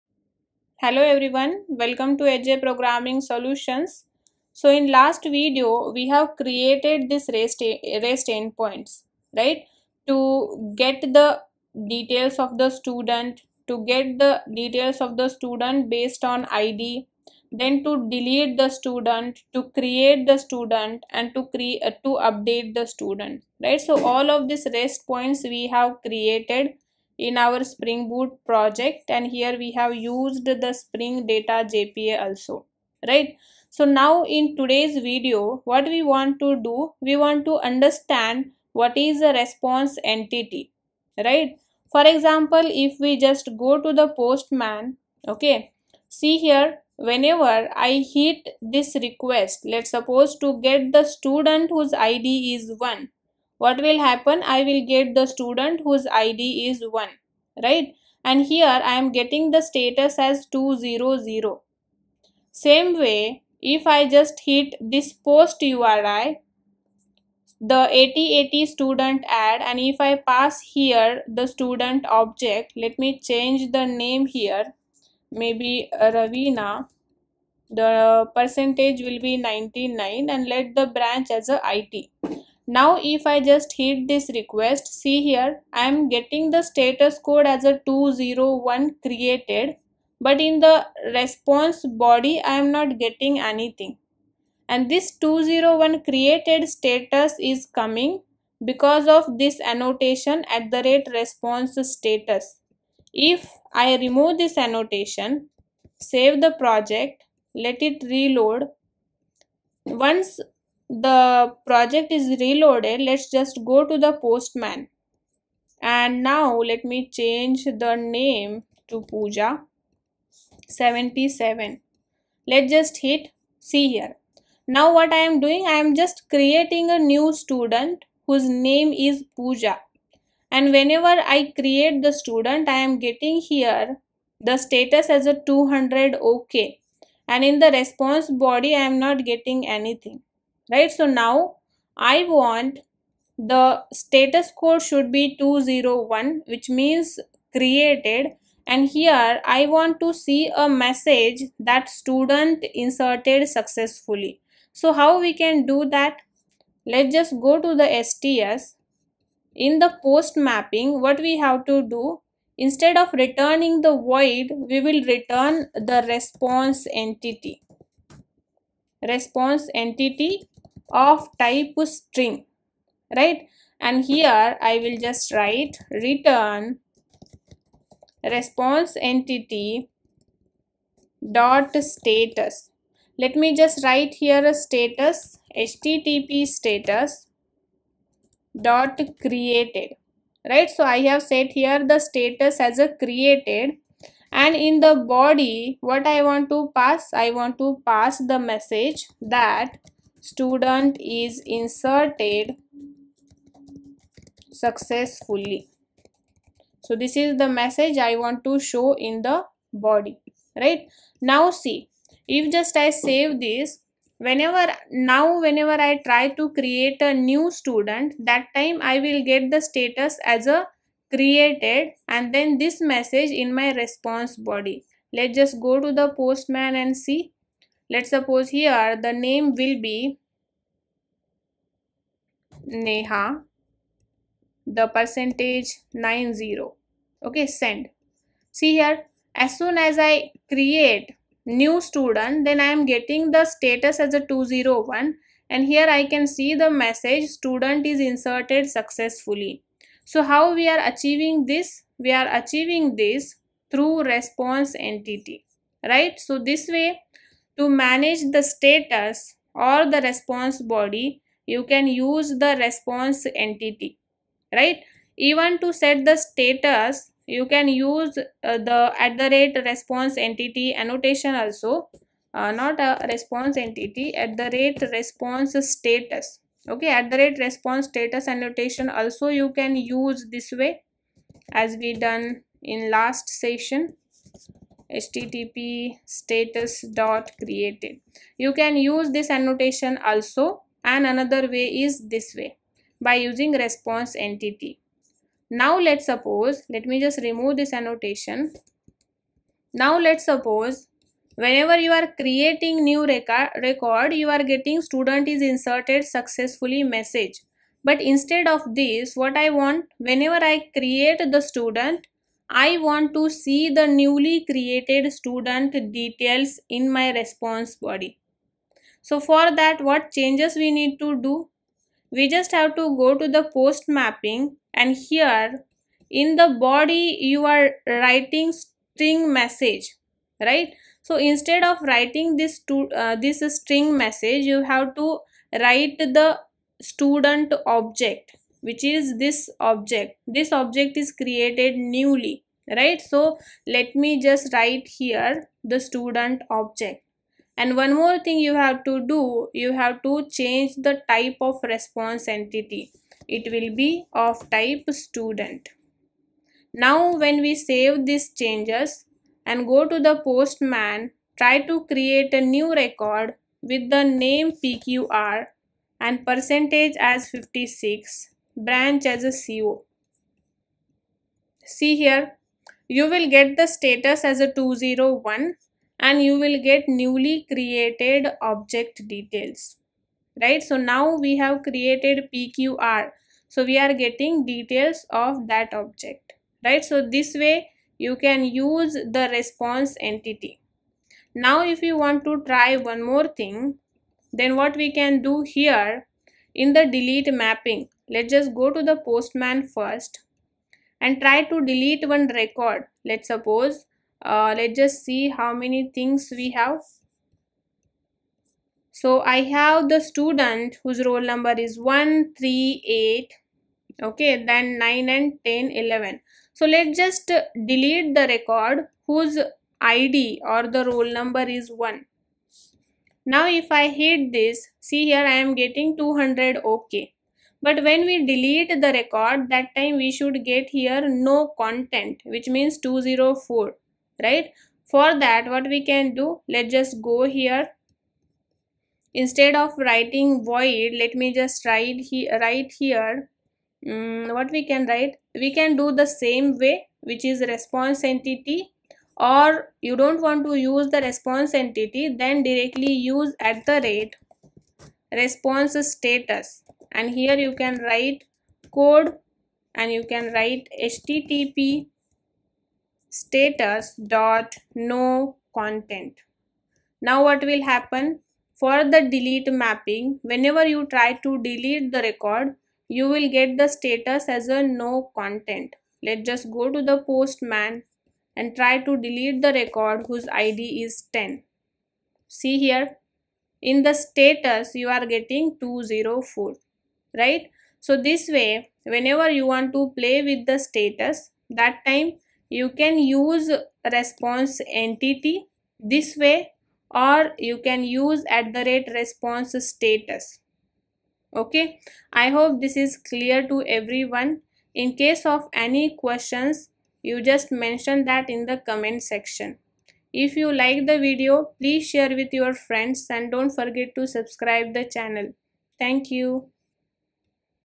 What is ResponseEntity in Spring Boot ?
How to change the status code in response using ResponseEntity ?
What is @ResponseStatus annotations used for ?